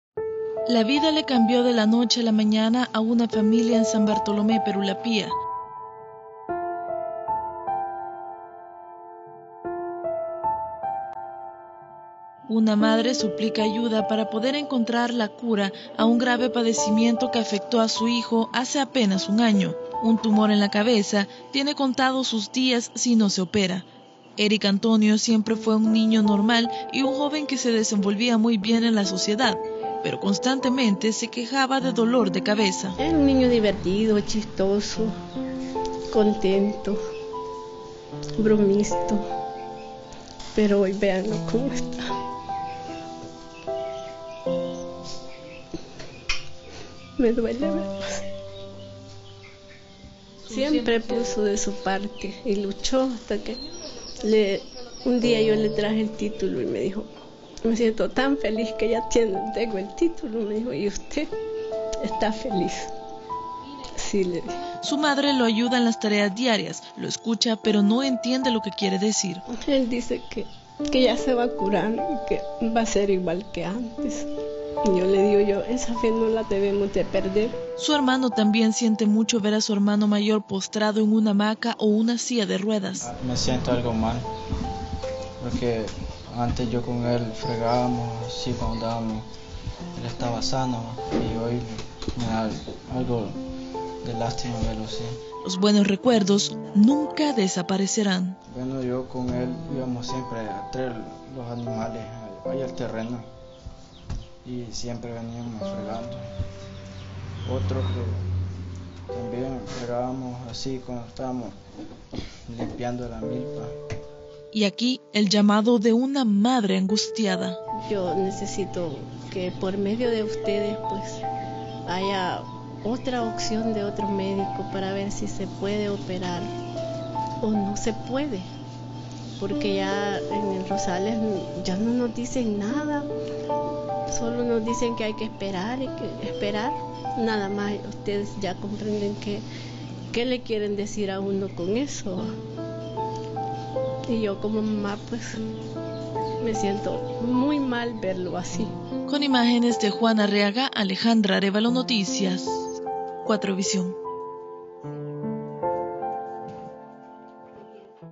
Voces de Ayuda Erick Antonio Alvarado Echeverría.
El futuro prometedor de un joven se vio truncado por una grave enfermedad. Su familia está desesperada y clama por ayuda para salvarle la vida. Números de contacto para colaborar: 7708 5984 //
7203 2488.